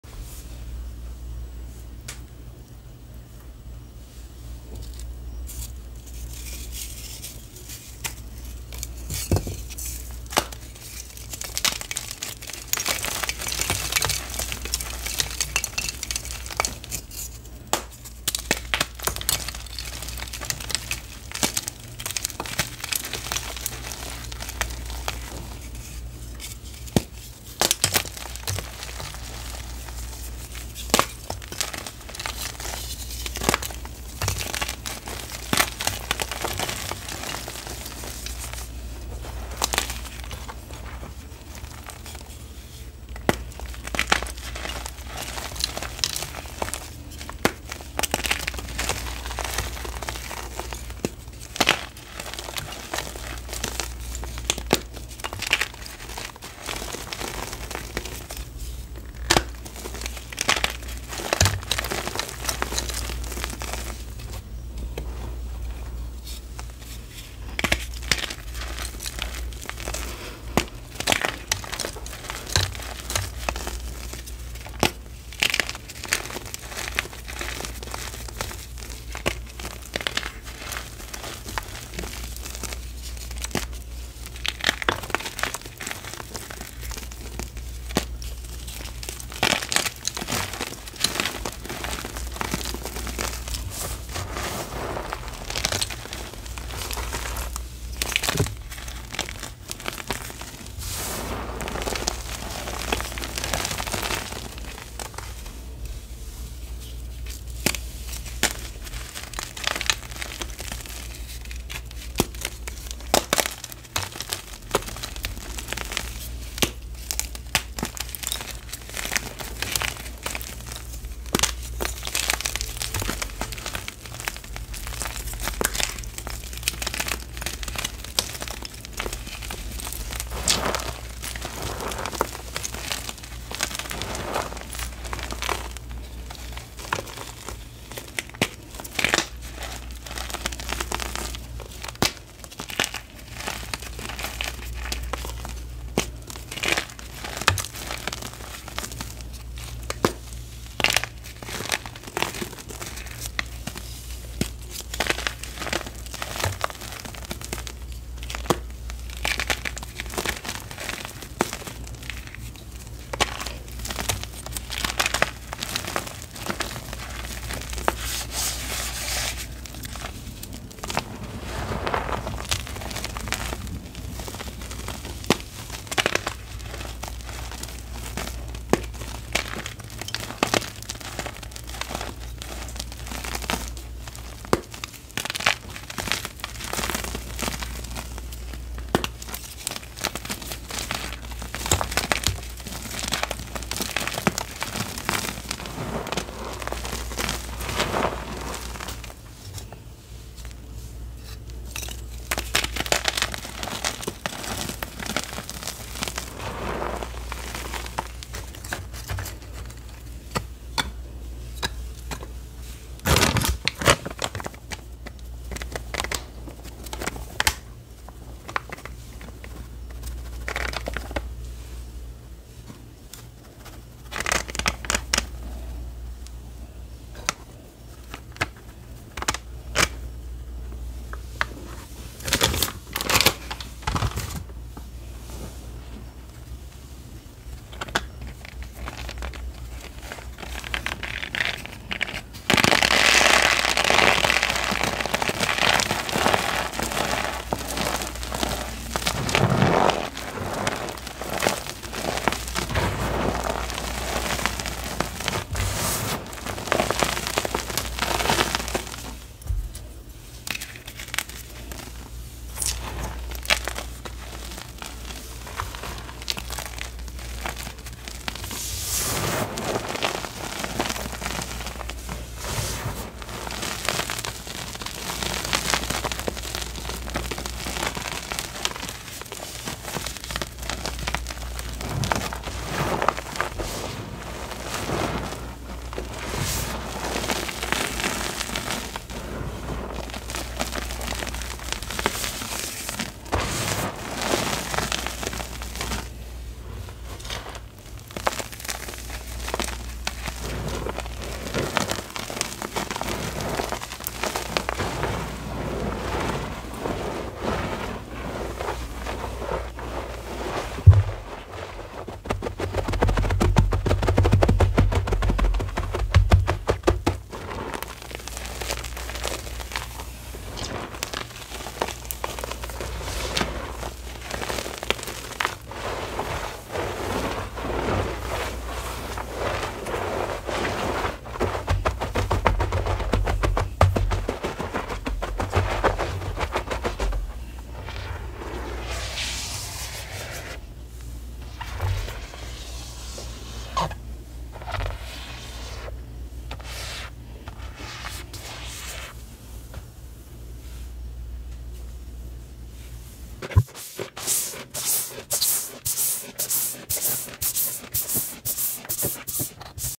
3 Fresh Blocks and 4 Acetone reforms with A CS Stack
I'll be out of town for a funeral until Monday at so I'll be squeezing in some more crushes before i leave. I just really want to thank everyone for the love. I'm glad you enjoy my channel and hope we have more fun to come!
enjoy!